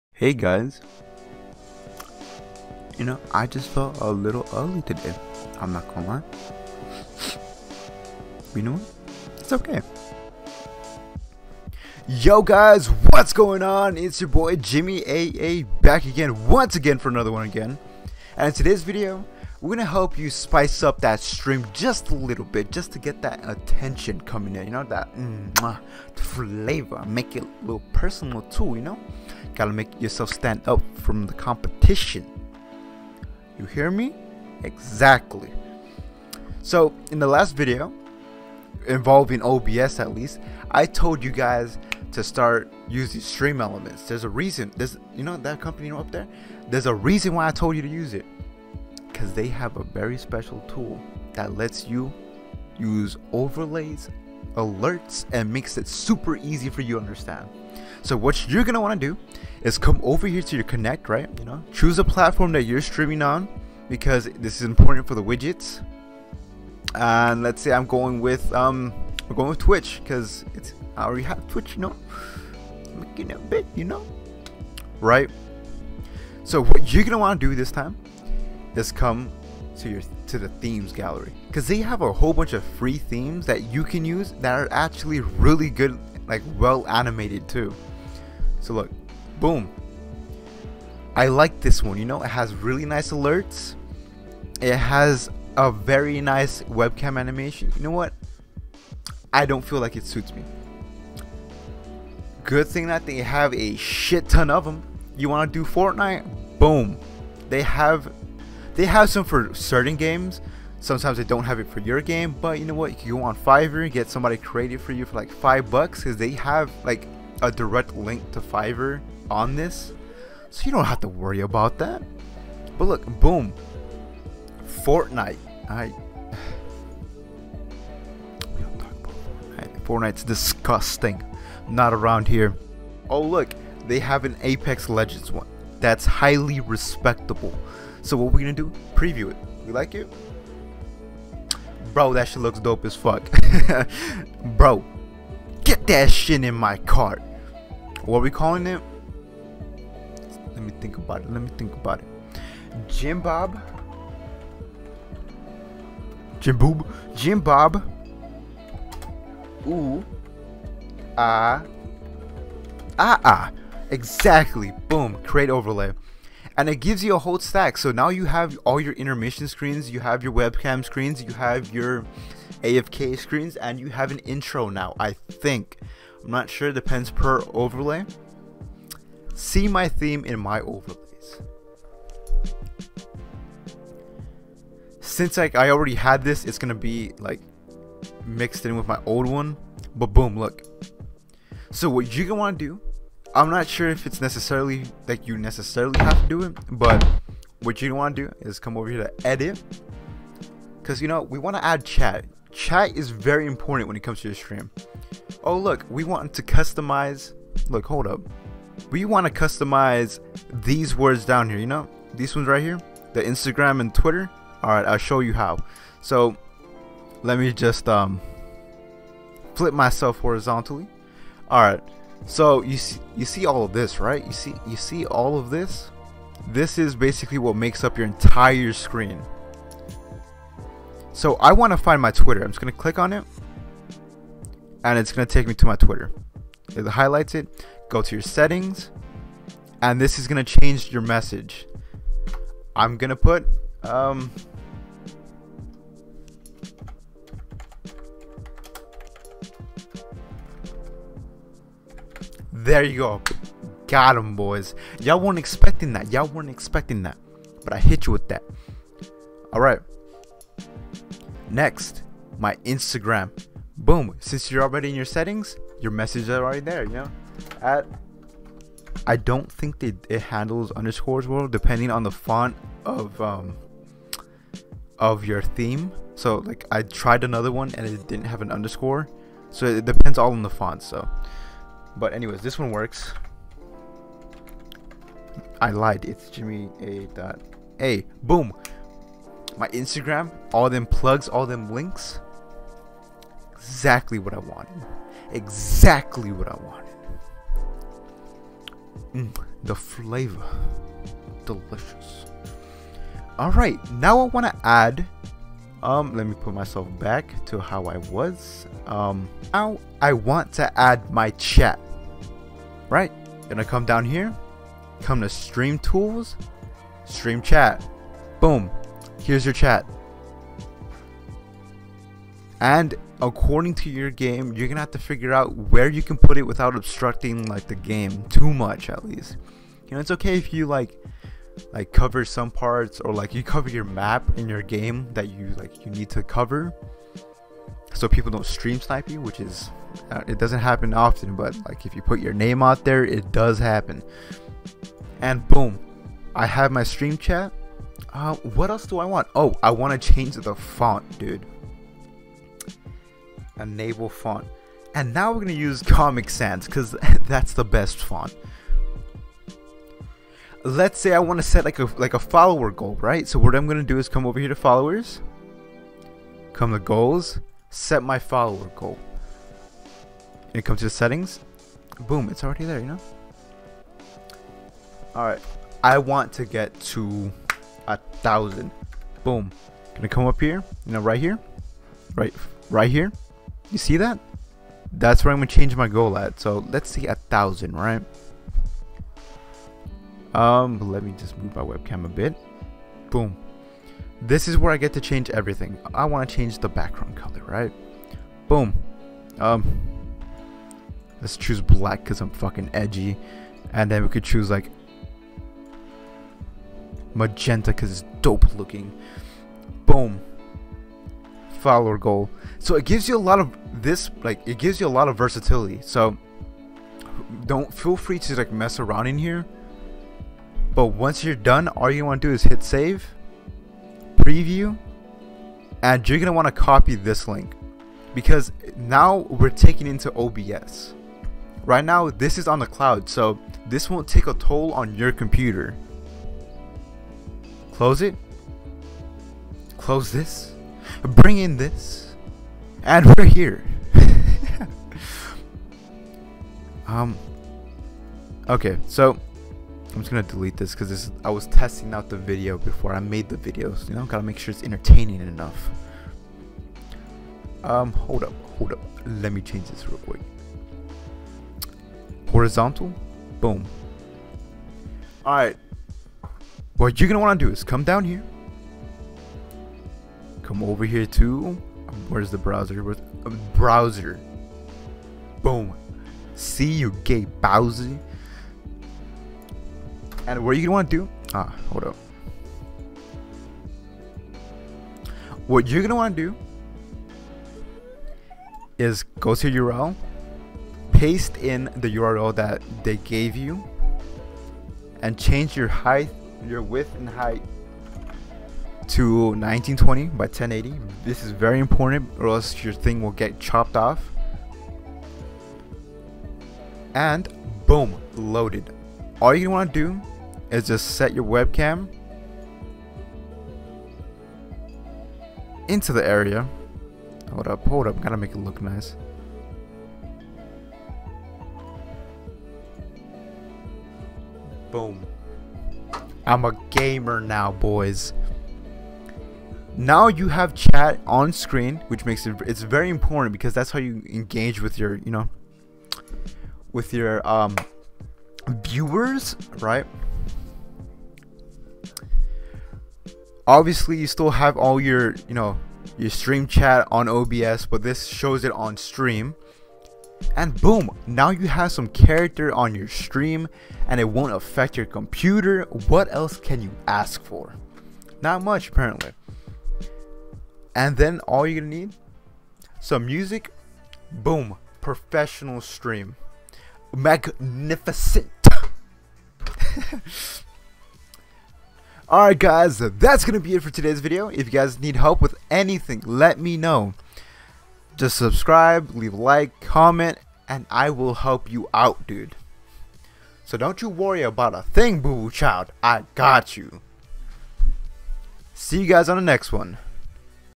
How to Make Your Stream Look PROFESSIONAL | Obs Tutorial
What's going on guys today I will be showing you how to setup a Webcam/stream overlay to make your stream pop-out from the rest of your competition and give your stream the professional look that you've been wanting to get.
Discord Server:
Twitch.tv/jimmyayeaye